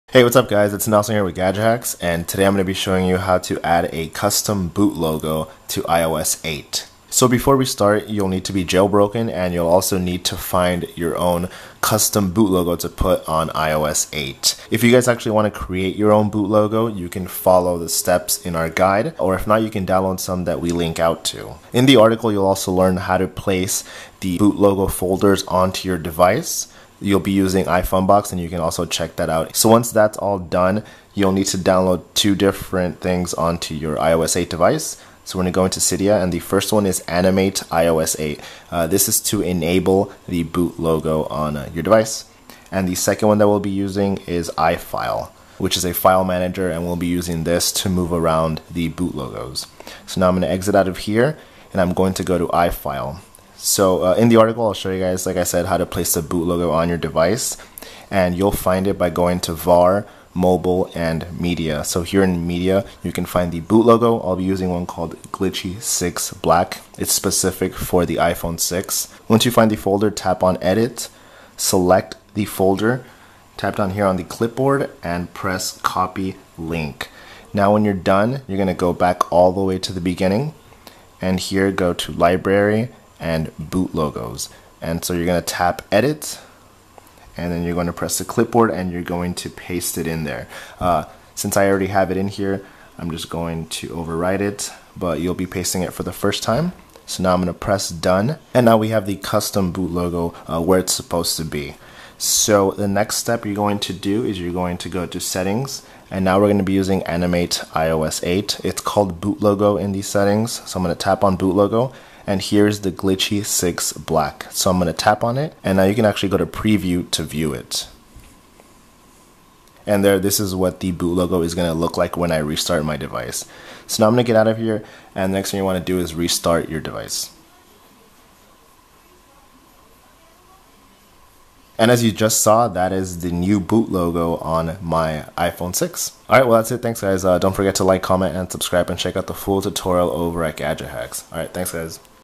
Change the Apple Boot Screen Animation on Your iPhone to Something Better [How-To]
How to Add Your Own Custom Boot Logo [iOS 8]

In this tutorial, I'll be showing you how to place your own custom boot logo on your iOS 8 device. For this guide you'll need to be jailbroken, as you'll be using a couple of Cydia tweaks.